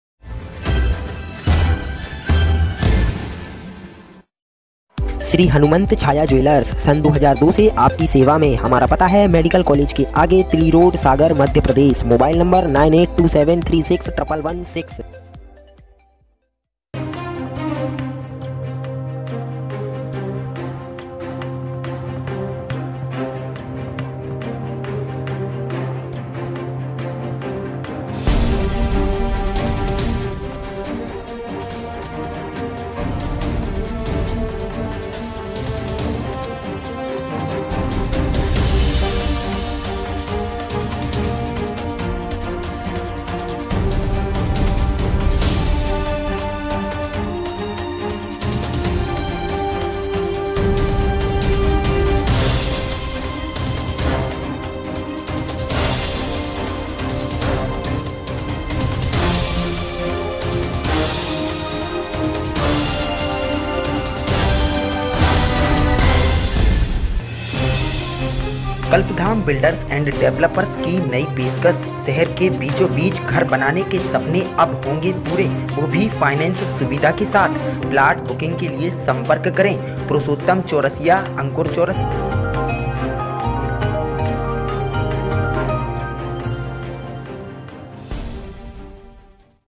BREAKING NEWS-सुरखी विधानसभा में मतदान प्रक्रिया शुरू-प्रशासन के पुख्ता इंतजाम
-सुरखी विधानसभा में मतदान प्रक्रिया शुरू-प्रशासन के पुख्ता इंतजाम